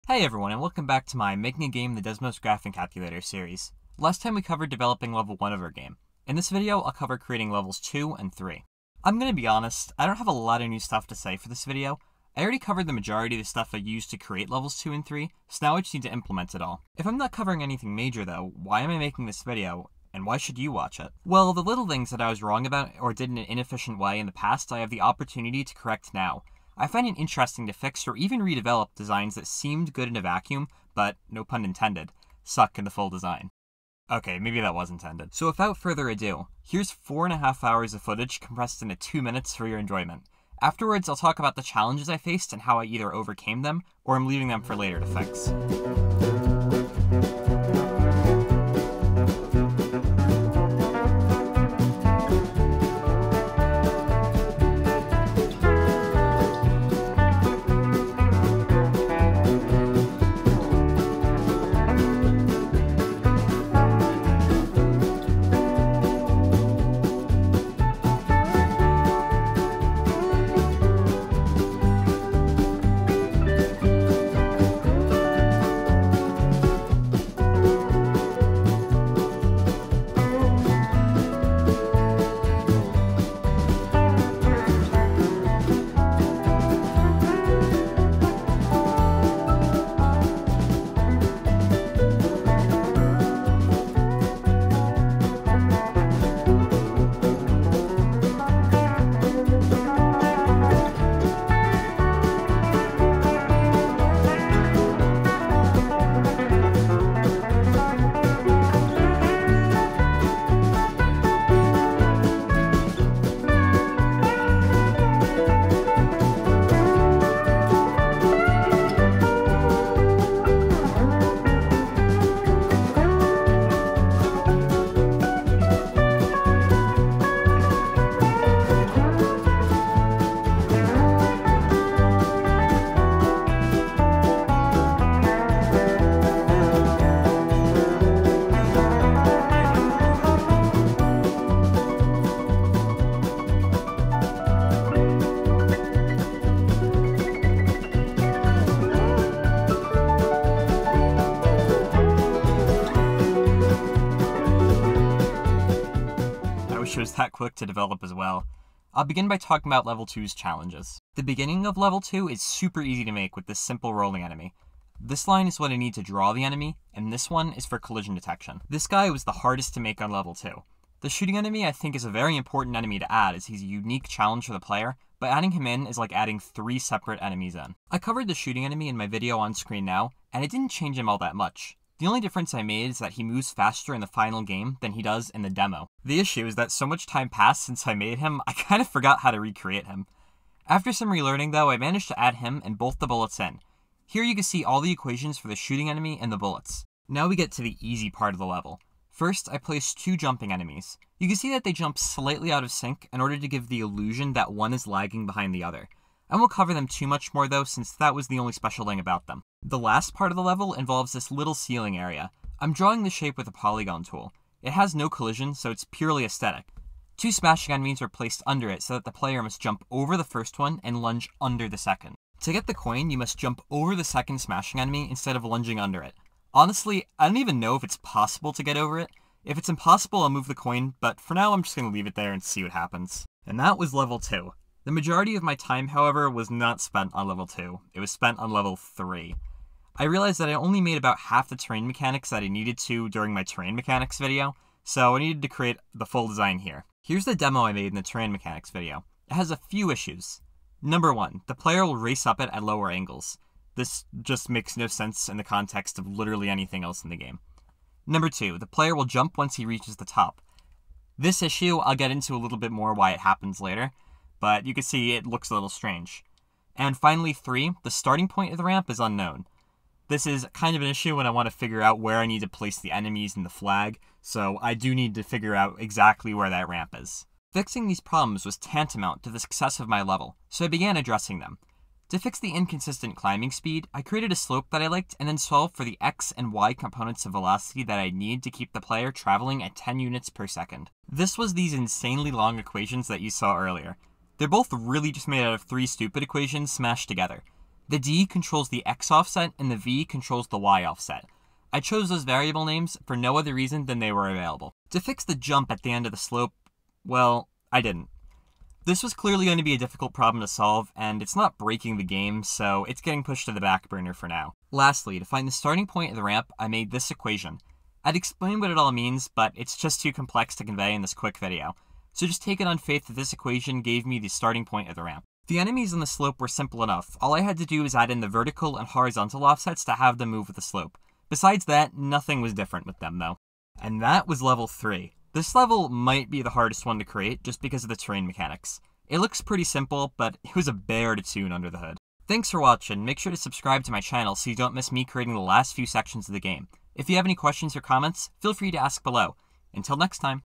Making a Game in Desmos: #8 - Levels 2 & 3
Please leave your feedback below!

In this video I cover developing the second and third levels of the game.